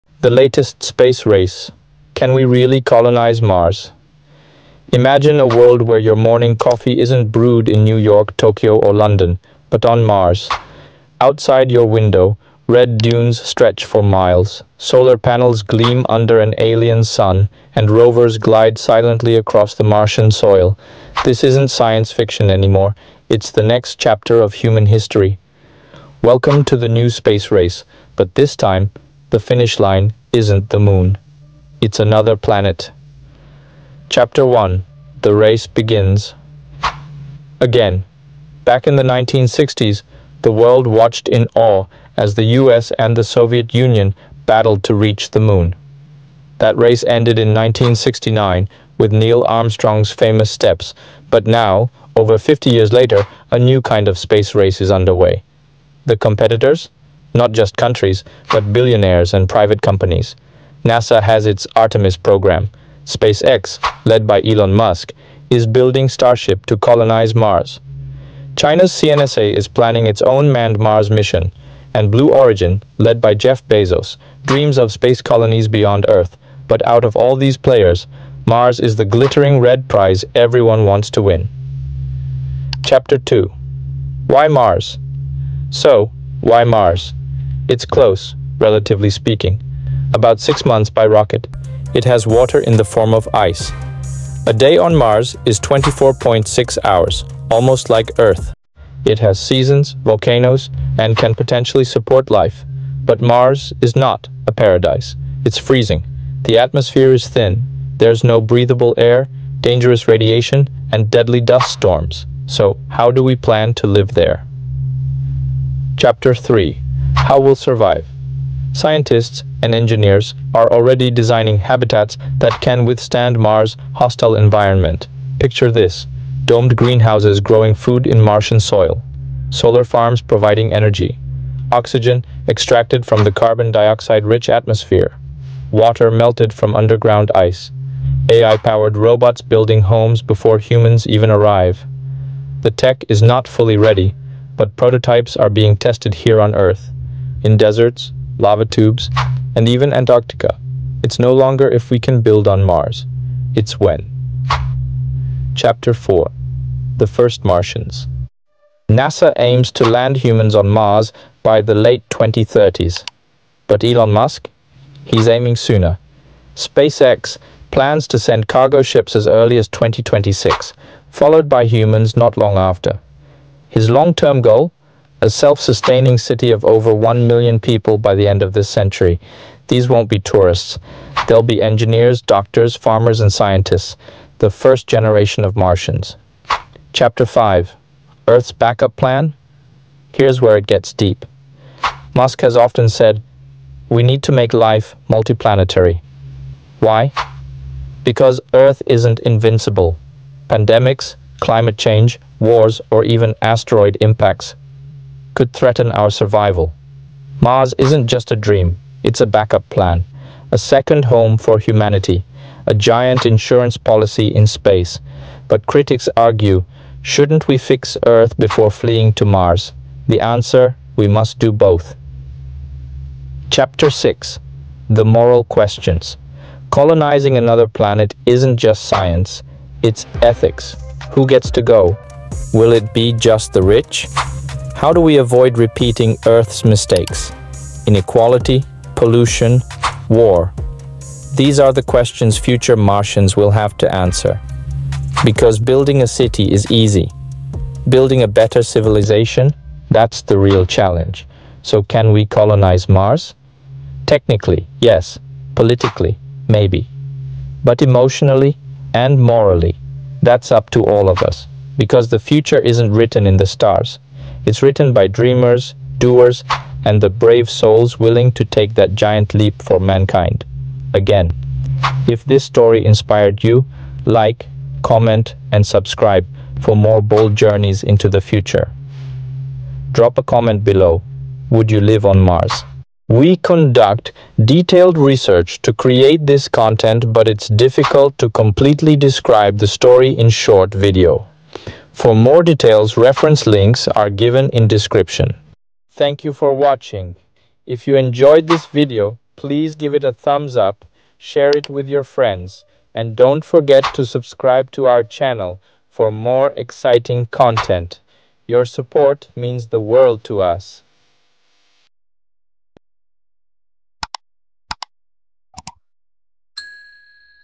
The Latest Space Race Can We Really Colonize Mars
Imagine a world where your morning coffee isn’t brewed in New York, Tokyo, or London—but on Mars.
Outside your window: red dunes stretch for miles, solar panels gleam under an alien sun, and rovers glide silently across the Martian soil.
This isn’t science fiction anymore—it’s the next chapter of human history.
Welcome to the new space race—but this time, the finish line isn’t the Moon…
It’s another planet.
Chapter 1: The Race Begins… Again
Back in the 1960s, the world watched in awe as the U.S. and the Soviet Union battled to reach the Moon. That race ended in 1969 with Neil Armstrong’s famous steps.
But now, over 50 years later, a new kind of space race is underway.
The competitors? Not just countries—but billionaires and private companies.
• NASA has its Artemis Program.
• SpaceX, led by Elon Musk, is building Starship to colonize Mars.
• China’s CNSA is planning its own manned Mars mission.
• And Blue Origin, led by Jeff Bezos, dreams of space colonies beyond Earth.
But out of all these players, Mars is the glittering red prize everyone wants to win.
Chapter 2: Why Mars?
So… why Mars?
✅ It’s close (relatively speaking)—about 6 months by rocket.
✅ It has water in the form of ice.
✅ A day on Mars is 24.6 hours—almost like Earth.
✅ It has seasons, volcanoes, and can potentially support life.
But Mars is not a paradise.
It’s freezing. The atmosphere is thin. There’s no breathable air, dangerous radiation, and deadly dust storms.
So how do we plan to live there?
Chapter 3: How We’ll Survive
Scientists and engineers are already designing habitats that can withstand Mars’ hostile environment.
Picture this:
• Domed greenhouses growing food in Martian soil
• Solar farms providing energy
• Oxygen extracted from the carbon dioxide-rich atmosphere
• Water melted from underground ice
• AI-powered robots building homes before humans even arrive
The tech is not fully ready, but prototypes are being tested here on Earth—in deserts, lava tubes, and even Antarctica.
It’s no longer if we can build on Mars. It’s when.
Chapter 4: The First Martians
NASA aims to land humans on Mars by the late 2030s.
But Elon Musk? He’s aiming sooner.
SpaceX plans to send cargo ships as early as 2026, followed by humans not long after. His long-term goal? A self-sustaining city of over one million people by the end of this century.
These won’t be tourists. They’ll be engineers, doctors, farmers, and scientists.
The first generation of Martians.
Chapter 5: Earth’s Backup Plan?
Here’s where it gets deep.
Musk has often said:
“We need to make life multiplanetary.”
Why?
Because Earth isn’t invincible. Pandemics, climate change, wars, or even asteroid impacts… could threaten our survival.
Mars isn’t just a dream—it’s a backup plan. A second home for humanity. A giant insurance policy in space.
But critics argue: Shouldn’t we fix Earth before fleeing to Mars?
The answer? We must do both.
Chapter 6: The Moral Questions
Colonizing another planet isn’t just science—it’s ethics.
• Who gets to go?
• Will it be just the rich?
• How do we avoid repeating Earth’s mistakes—inequality, pollution, war?
These are the questions future Martians will have to answer.
Because building a city is easy.
Building a better civilization? That’s the real challenge.
[OUTRO – Call to Action]
So, can we colonize Mars?
Technically—yes.
Politically—maybe.
But emotionally and morally—that’s up to all of us.
Because the future isn’t written in the stars.
It’s written by dreamers, doers, and the brave souls willing to take that giant leap for mankind… again.
🌌 Would YOU live on Mars?